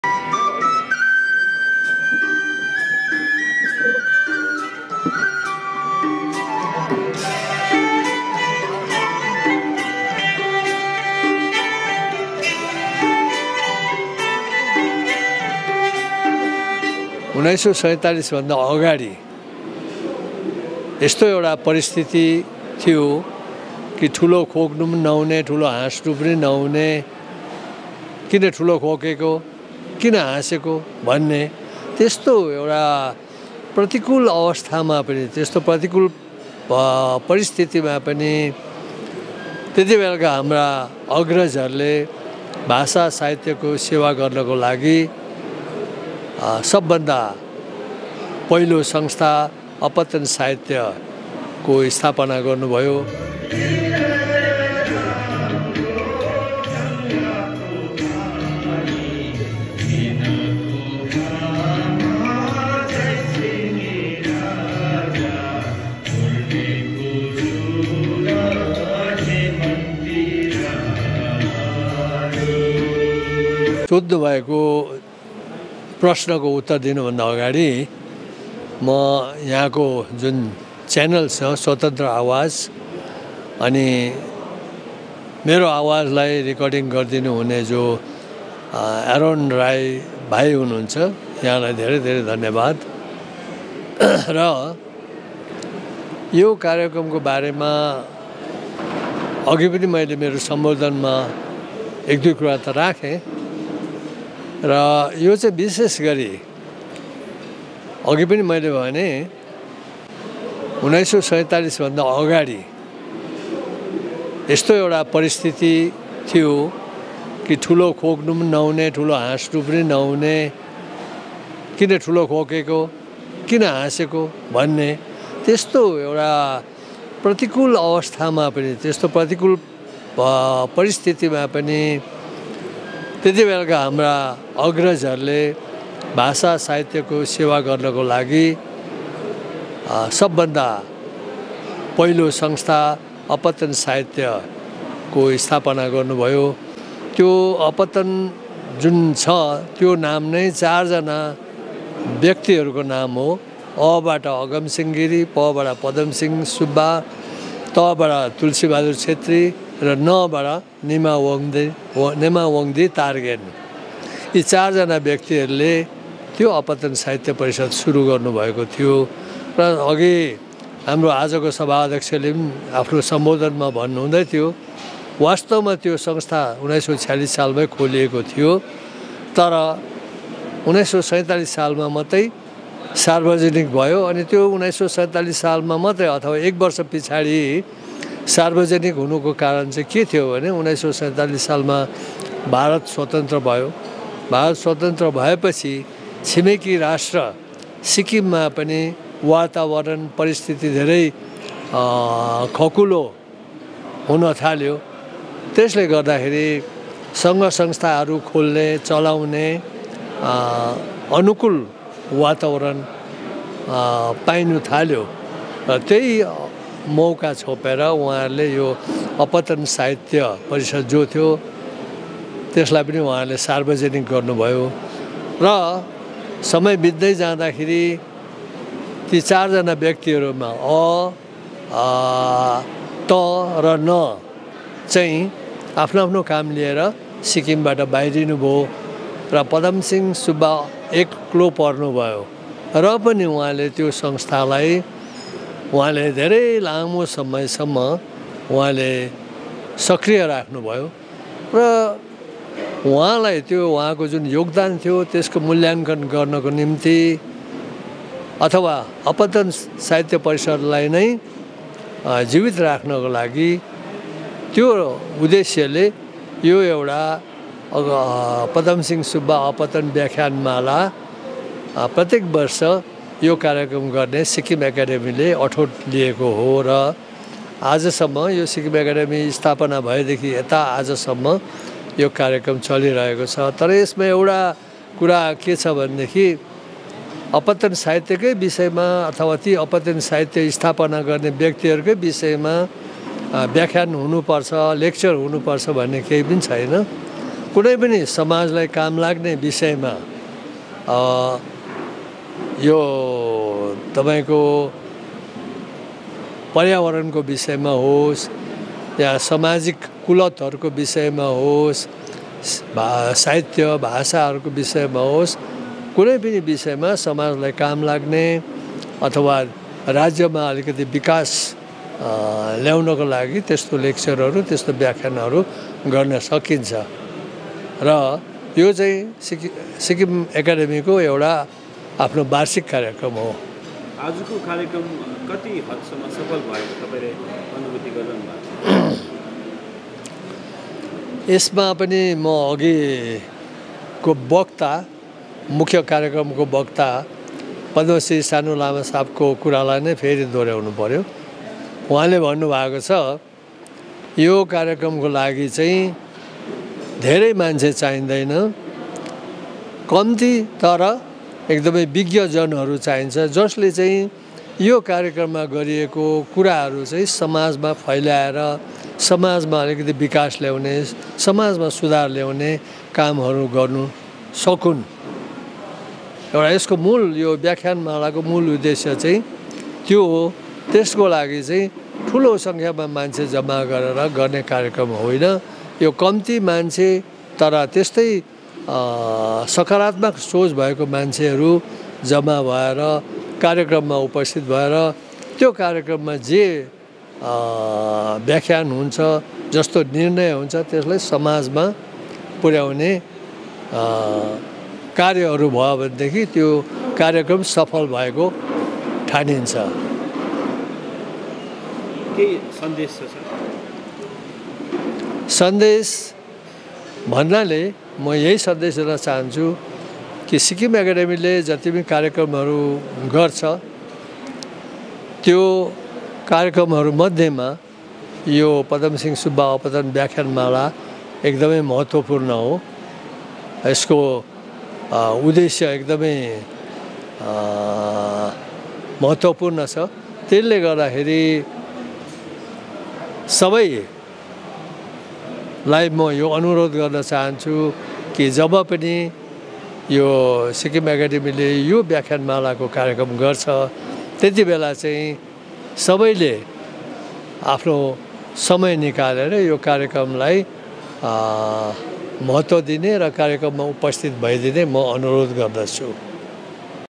Sikkim Akademi today held the Eighth Padam Singh Subba ‘Apatan’ talk series 2024 programme at Gtk
Sikkim Akademi today held the Eighth Padam Singh Subba ‘Apatan’ talk series 2024 programme at Tashi Namgyal SSS, Development Area in Gangtok.  Culture minister GT Dhungel was the chief guest for the event which was chaired by Padmashree Sanu Lama and attended by distinguished litterateurs and invitees.